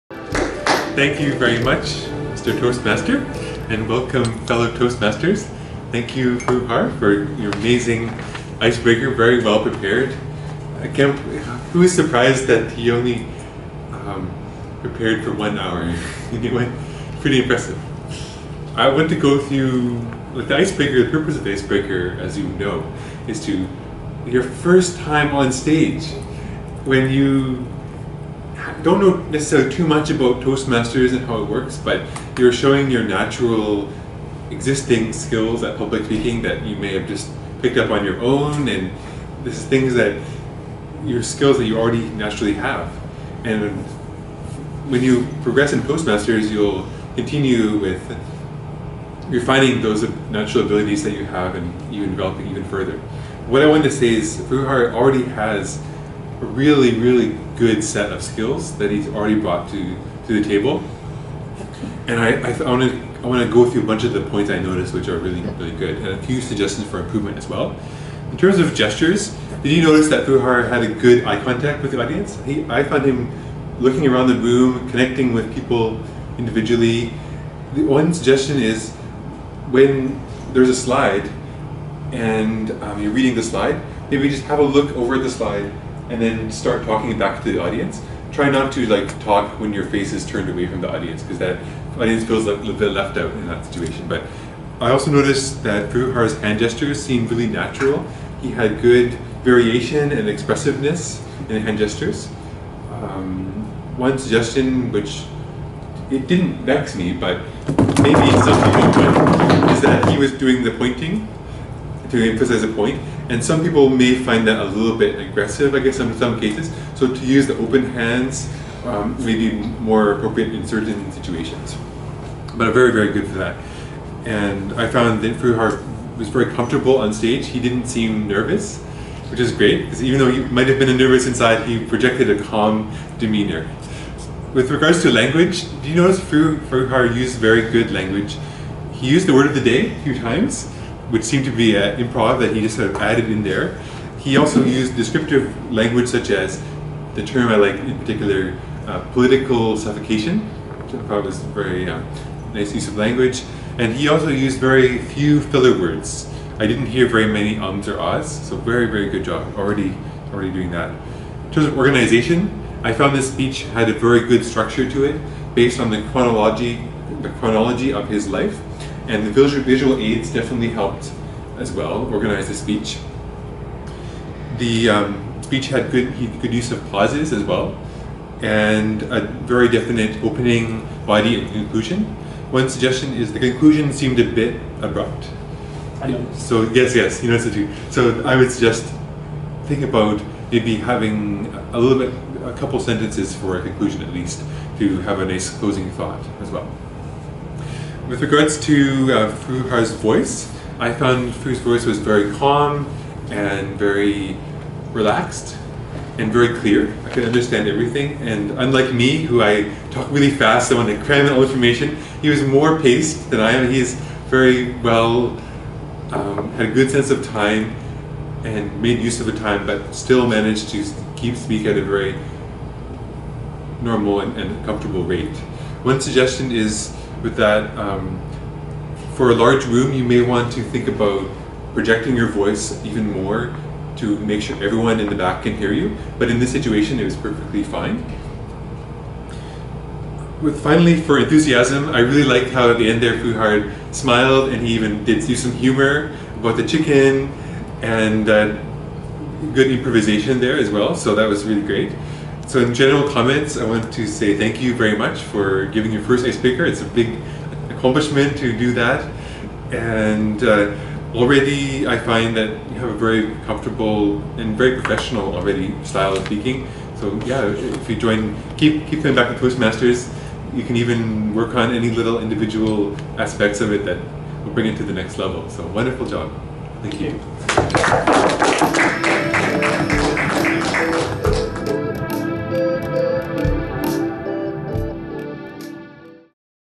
Toastmasters Speech Evaluation sample by Geoff Peters - Feb 2013
gives a speech evaluation at his Toastmasters public speaking club.

Thanks!!!

In Toastmasters, every speaker is evaluated. An evaluator provides helpful feedback on what the speaker did well and some suggestions for improvement.

Presented at Best Run Toastmasters Club in Vancouver BC Canada on Feb 14th 2013.

Toastmasters has really helped me improve my communications skills. It will work for you too, and clubs are all around the world. I highly recommend checking out a local club in your area!

Cheers,
Geoff

Filmed on a Sony Cybershot DSC-HX30v and edited with Adobe Premiere Pro CS5.5 on a Windows 7 Dell PC (Intel i7 and USB 3.0 for a faster workflow).

Original music by Laundryman (a Vancouver electronic music band featuring Geoff Peters and Jason Knight), used with permission.